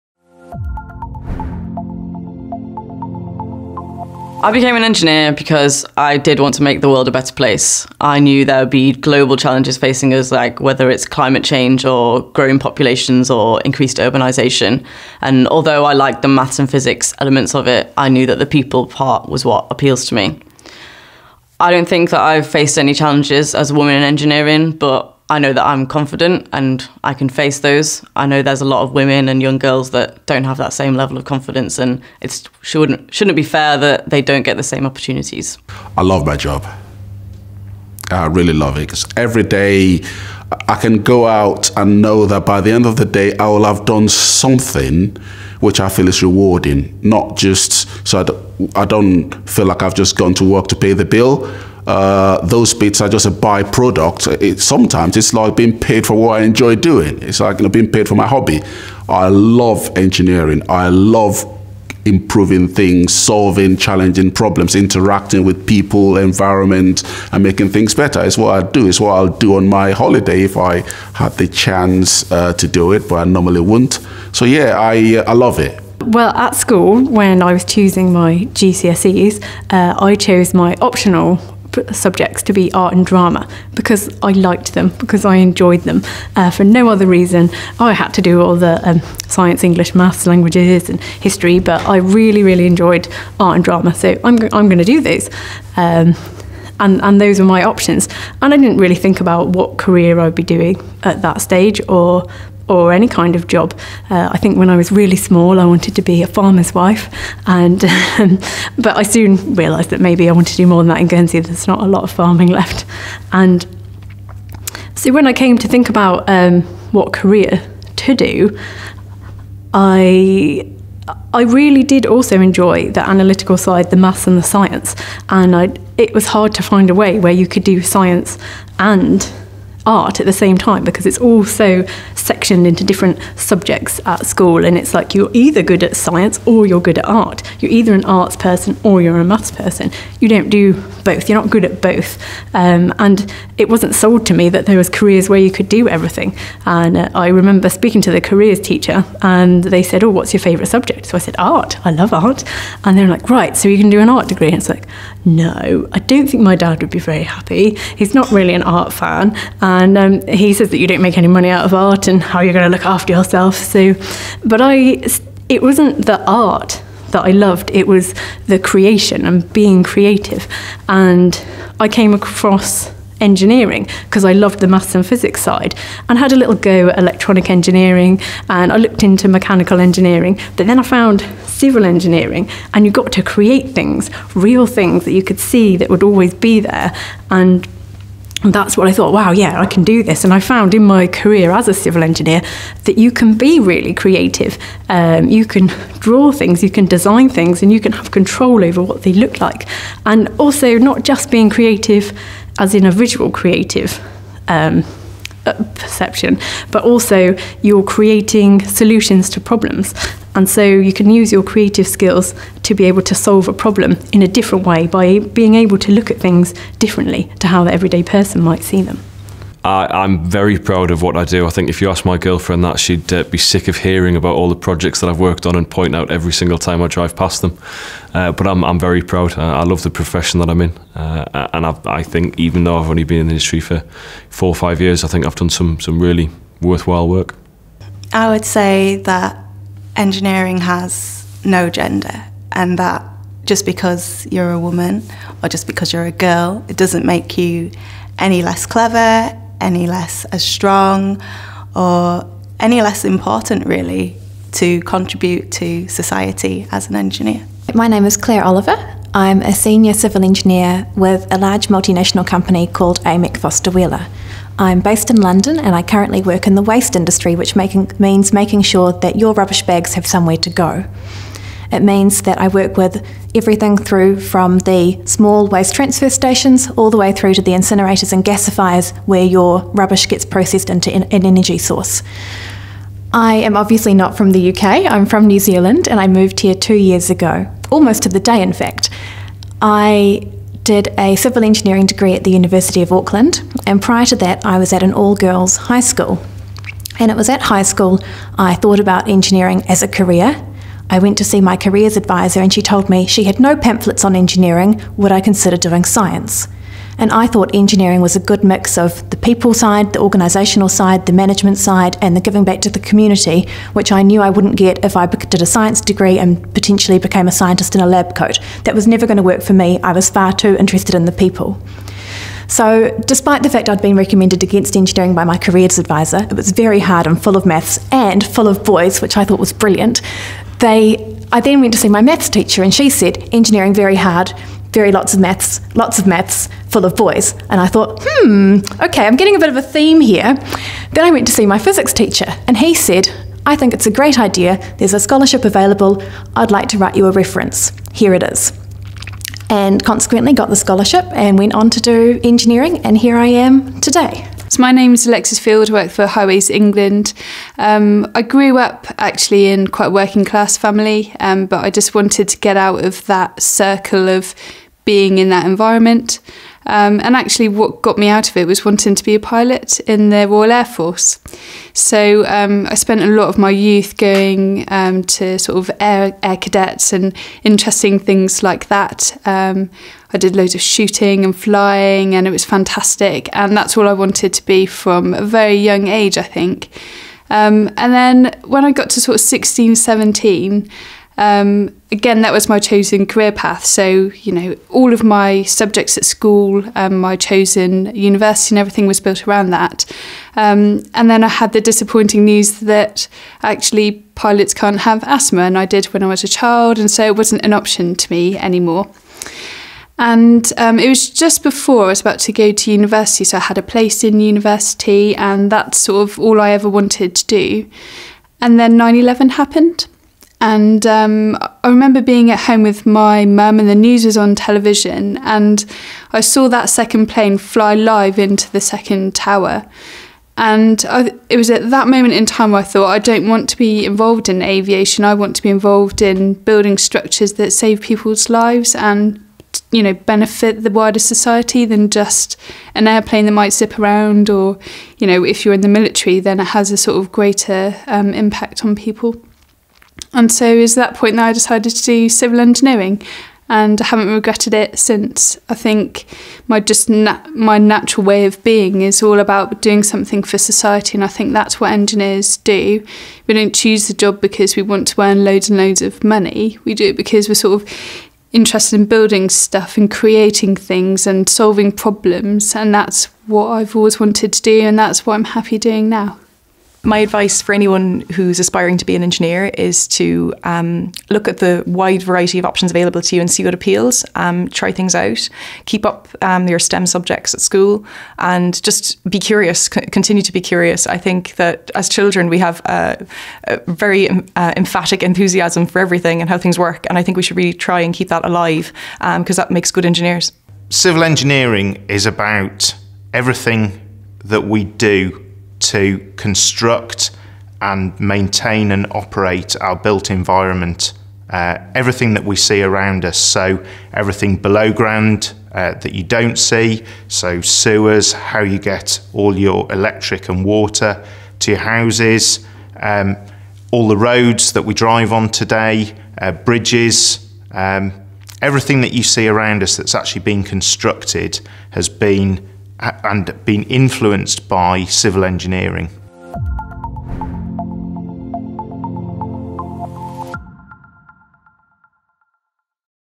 ICE Talks: Why become an engineer?
This ICE Talks resource features a number of members telling us why they became civil engineers and why it is such a fantastic career!

If you would like to explore a career in Civil Engineering